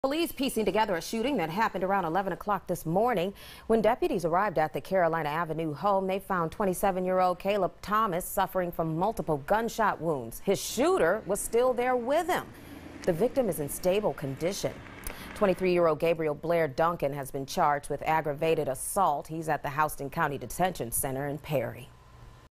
Man face aggravated assault charge in Warner Robins shooting
Warner Robins Police are piecing together a shooting that happened around 11:00 Wednesday morning. When deputies arrived at the Carolina Avenue home, they found 27-year-old Kalup Thomas suffering from multiple gunshot wounds. His shooter was there with him.